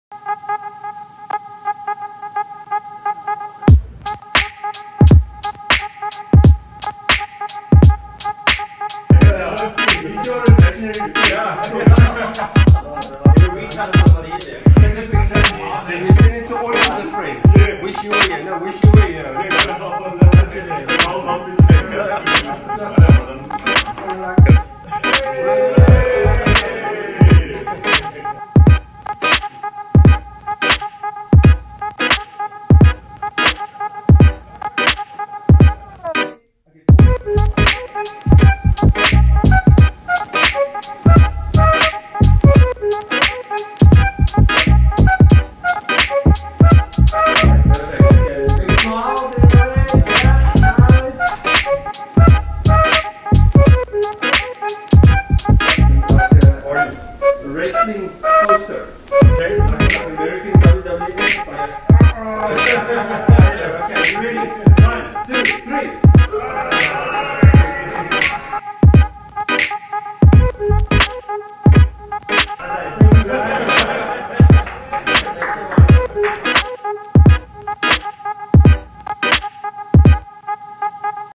BTS (Weddings) - Shooting the Boys
What happens at a wedding stays at a wedding... Well, maybe not. Here is a little sneak behind the scenes.
As guests we only see one part of the wedding.
What is it like to step behind the scenes and see what goes on “backstage”?

In this Episode we look at what goes on behind the scenes with shooting the Boys/entourage/Brothers/Groomsman.

It can sometime become a bit rowdy. So to keep all the boys on track can be interesting. My job is to capture this time accurately and have SOME FUN with the boys.

* If you are a bride/groom (old or new), or in the industry would love to hear your thoughts.
** If you know of a future bride or groom to be, please tag them in the comments (or share). They might just find this useful